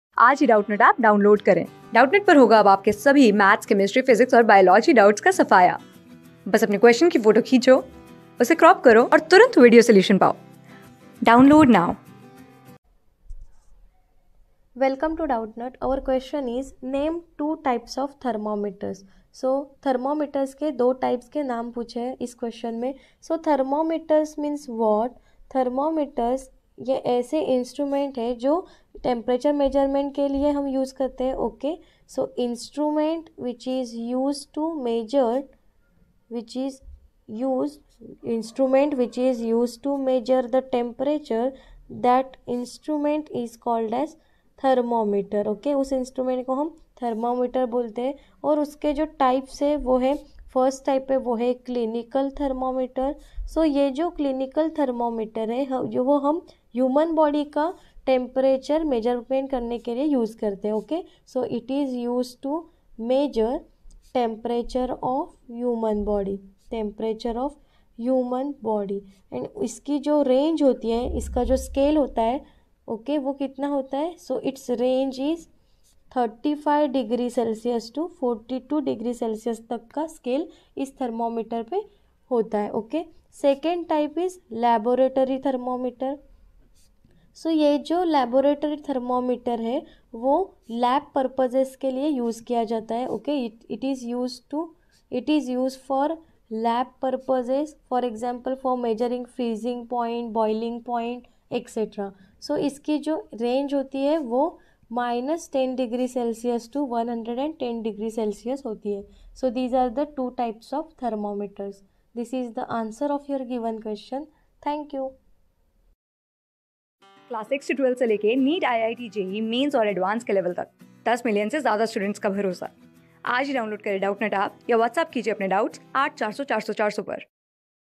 Name two types of thermometers. | CLASS 7 | HEAT | PHYSICS | Doubtnut
Name two types of thermometers.
Class: 7
Subject: PHYSICS
Chapter: HEAT
Board:Haryana Board

👉 Have Any Query? Ask Us.